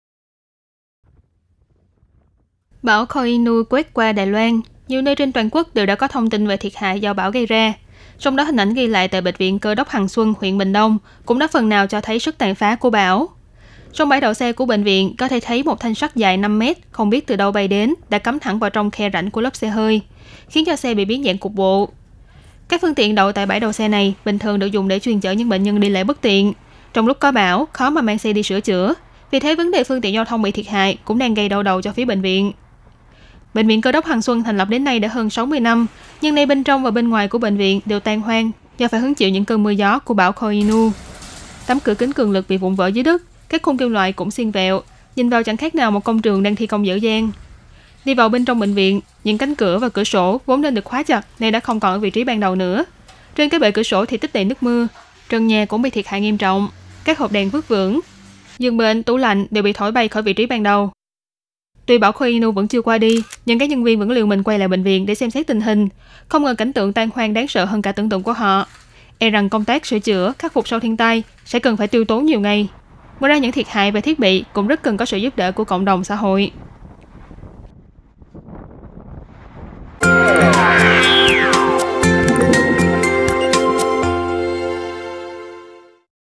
Bão Koinu gây thiệt hại tại Bệnh viện Cơ Đốc Hằng Xuân
Bão Koinu quét qua Đài Loan, nhiều nơi trên toàn quốc đều đã có thông tin về thiệt hại do bão gây ra, trong đó hình ảnh ghi lại tại Bệnh viện Cơ Đốc Hằng Xuân, huyện Bình Đông cũng đã phần nào cho thấy sức tàn phá của bão.

Bão Koinu vẫn chưa qua đi, nhưng các nhân viên vẫn liều mình quay lại bệnh viện để xem tình hình, không ngờ cảnh tượng tan hoang đáng sợ hơn cả tưởng tượng của họ, e rằng công tác sửa chửa, khắc phục sau thiên tai sẽ cần phải tiêu tốn nhiều ngày, ngoài ra những thiệt hại về thiết bị cũng rất cần có sự giúp đỡ của cộng đồng, xã hội.

Đài Phát thanh Quốc tế Đài Loan (Radio Taiwan International)
Ban Việt Ngữ
Lịch phát sóng chương trình
Inbox: m.me/banvietnguRTITaiwan